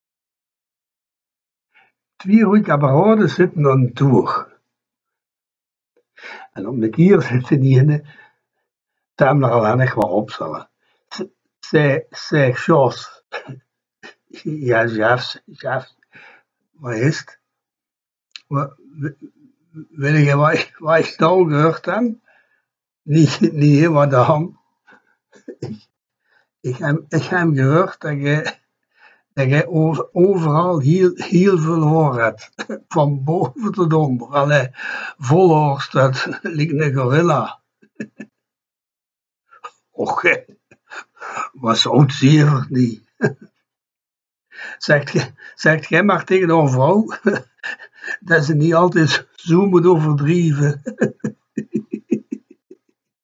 gorilla
twee goede kameraden zitten aan de toog. Een mop in het sappig dialect van Pelt, in Dommelland. Volkse humor met een knipoog naar het leven.